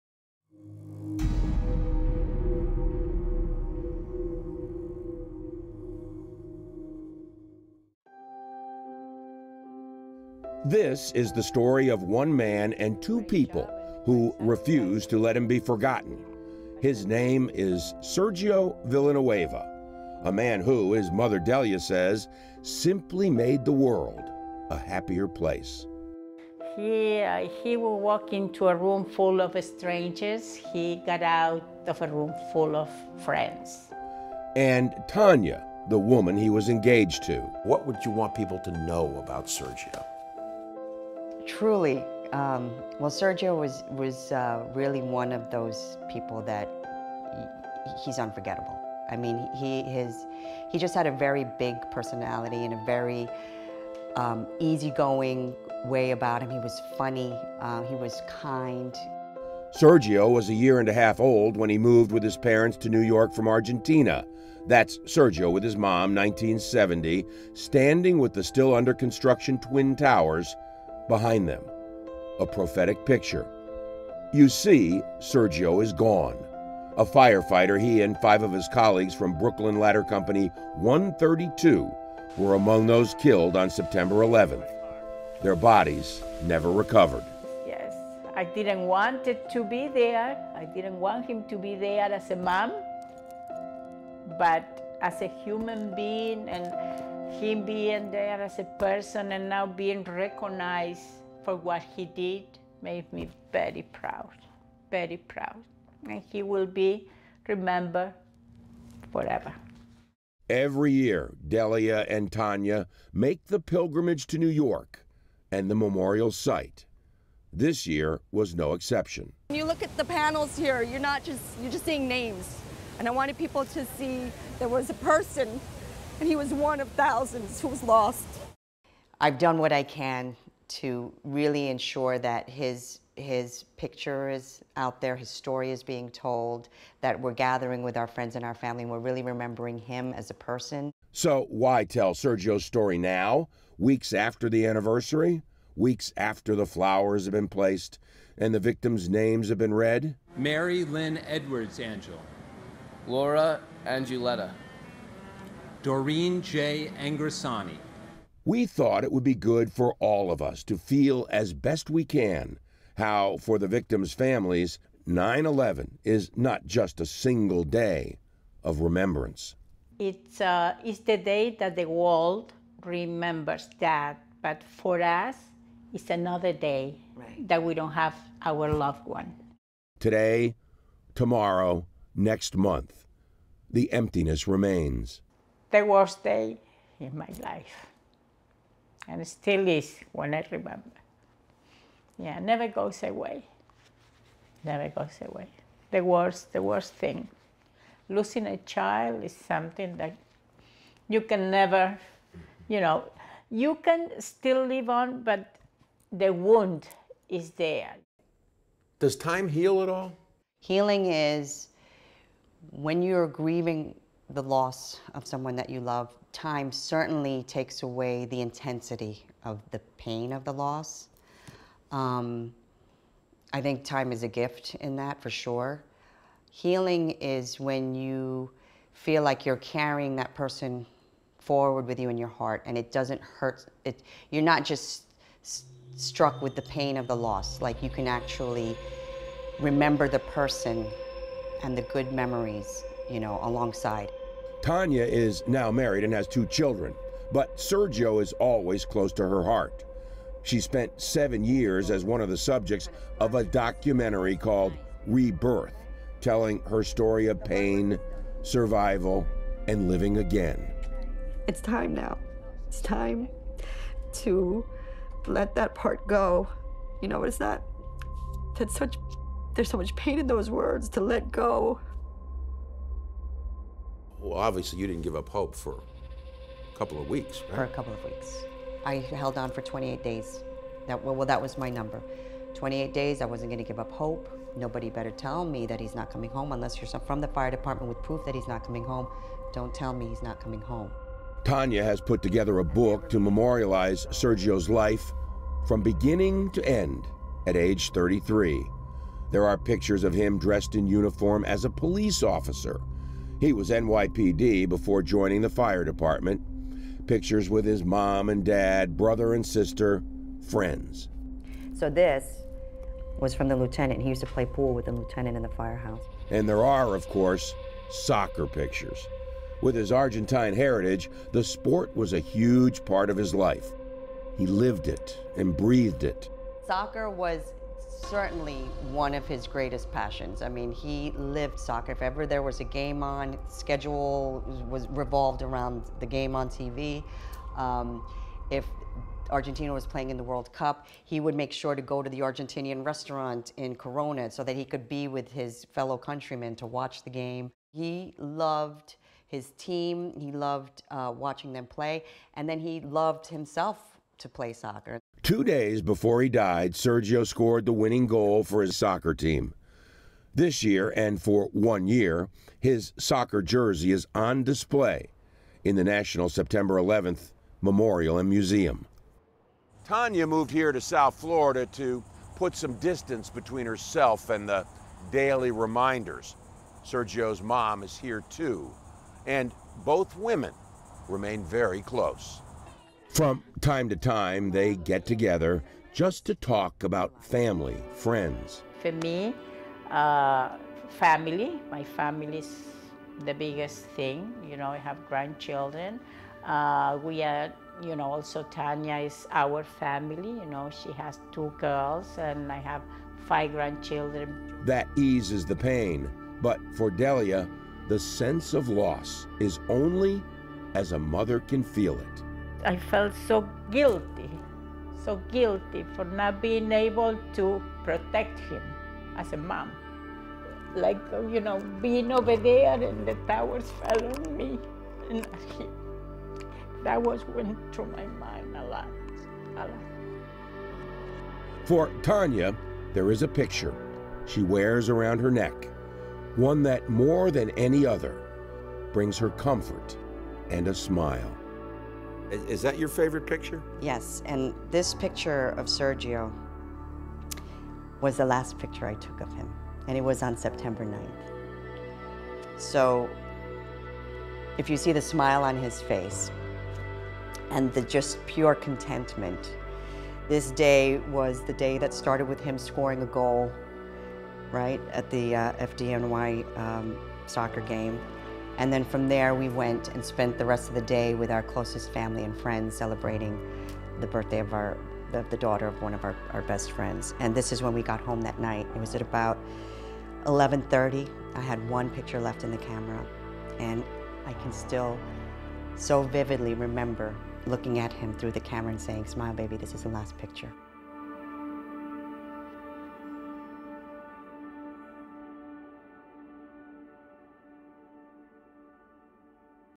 Keeping the memory of a 9-11 firefighter alive every day
The destruction of New York City’s World Trade Center in 2001 is observed each year with a ceremony at the site on September 11th - the anniversary of the attack.

But the family of one New York firefighter who died in the Twin Towers, has made it their duty to honor his memory not just on 9-11 but every day.

Correspondent John Zarrella talks to the mother and former fiancé of Sergio Villanueva and reports on their labor of love to preserve his larger-than-life legacy.

By the way, Sergio’s fiancé, Tanya, took his name after he died.